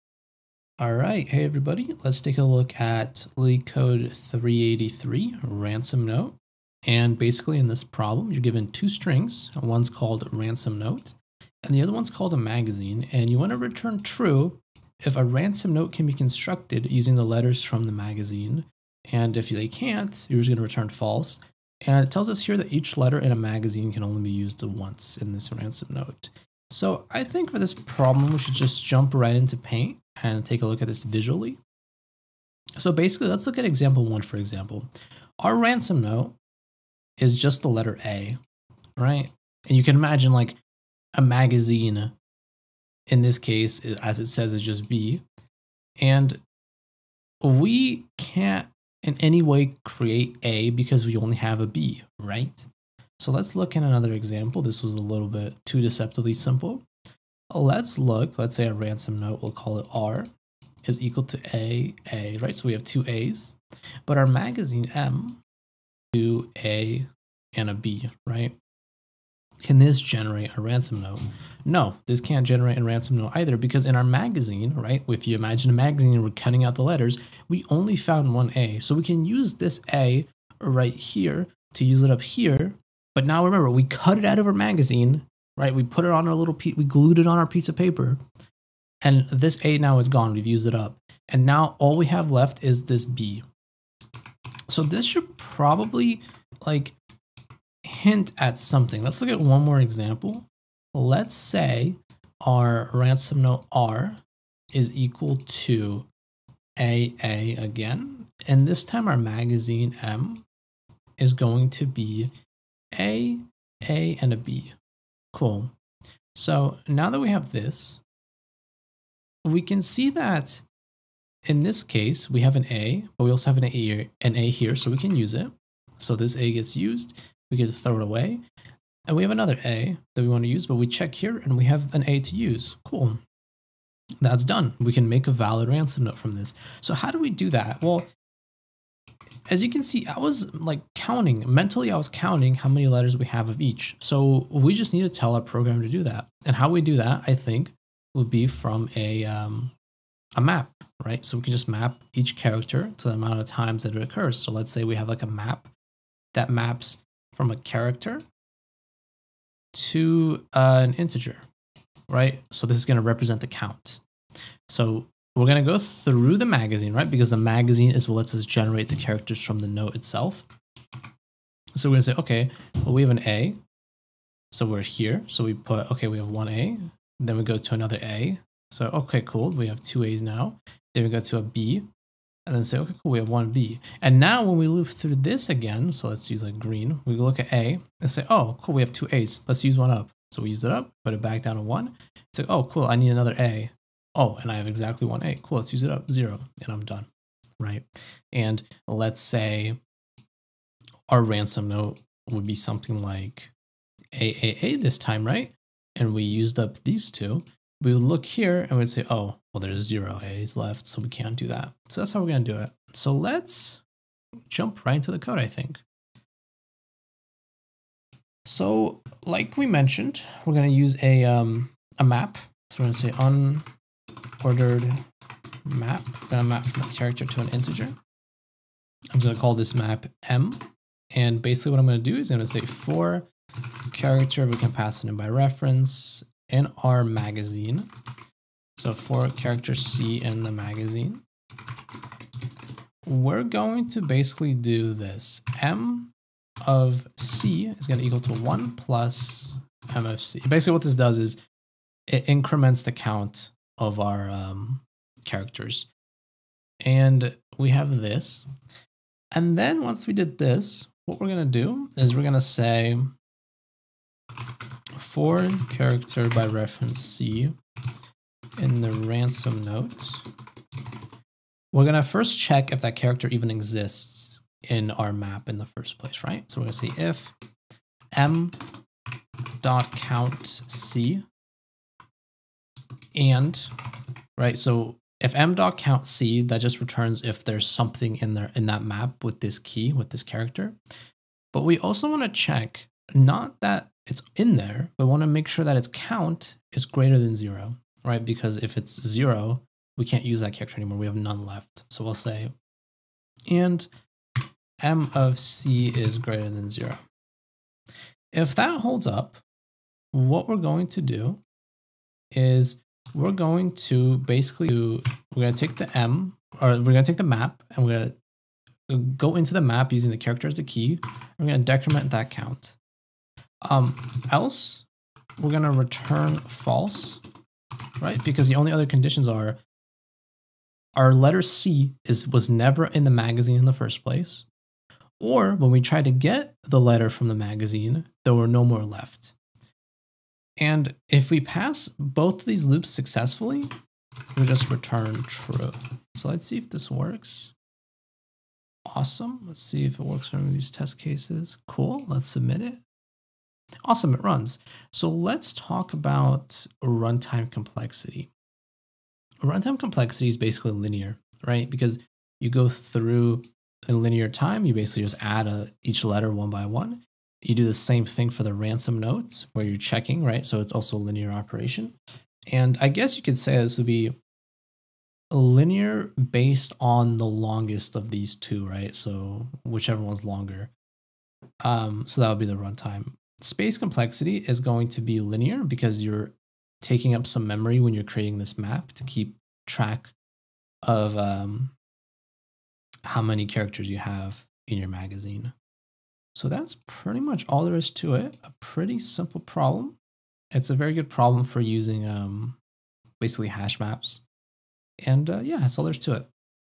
LeetCode 383 - Ransom Note - C++
This video is an explanation of the optimal run-time solution for LeetCode problem 383 - Ransom Note.

Timestamps:
00:00 - Problem Description
03:37 - Solution Code